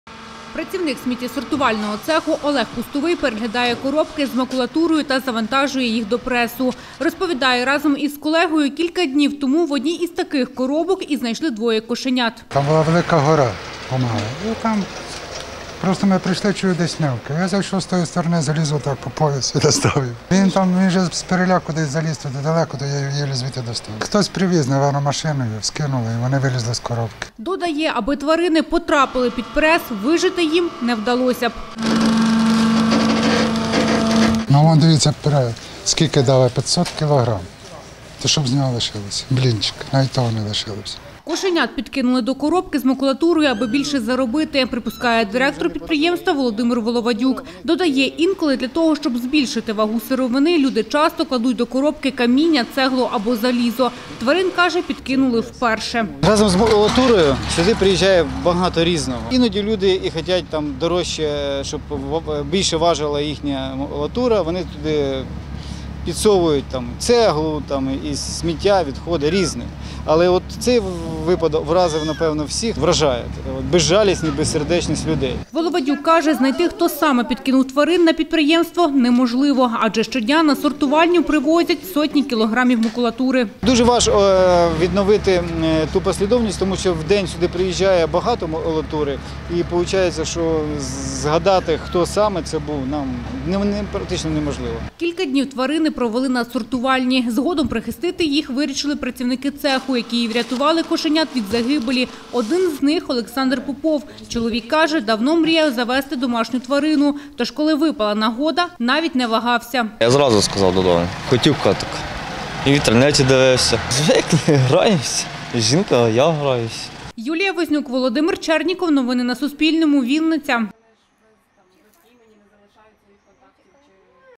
Хотіли заробити більше. На сортувальні у Вінниці підклали кошенят до макулатури
Двоє кошенят в коробках з макулатурою знайшли на вінницькому підприємстві з сортування вторсировини. Тварини ледве не потрапили під кілкасоткілограмовий прес,  Про це Суспільному розповів працівник цеху Олег Пустовий. Каже писк тварин  почули випадково. До поліції не зверталися, бо невідомо чи їх туди підкинули, для збільшення ваги макулатури, чи кошенята до коробки залізли самі. Обом вже  знайшли домівки. Подробиці далі у сюжеті.